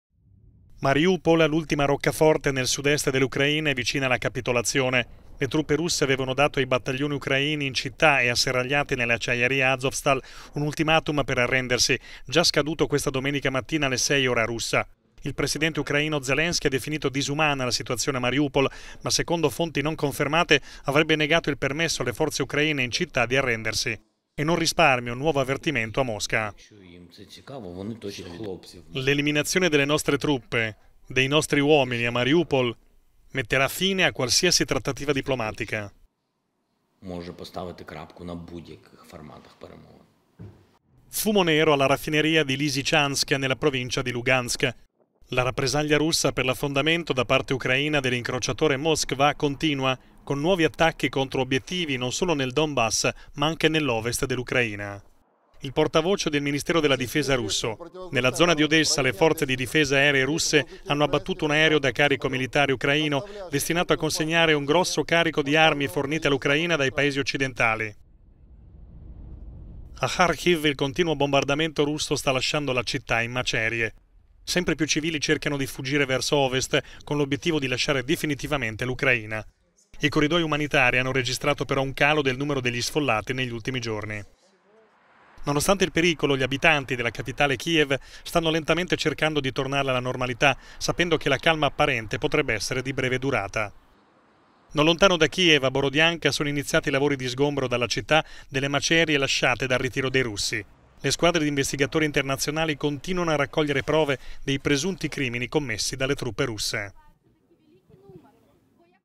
Ucraina, 53° giorno di guerra: battaglia finale per Mariupol e per l'acciaieria Azovstal
Scaduto l'ultimatum delle forze russe per la resa degli ucraini a Mariupol, in città e asserragliati nell'acciaieria Azovstal. Zelensky avrebbe vietato loro di arrendersi e minaccia la fine dei negoziati con Mosca. Russi pronti a sbarcare al porto di Mariupol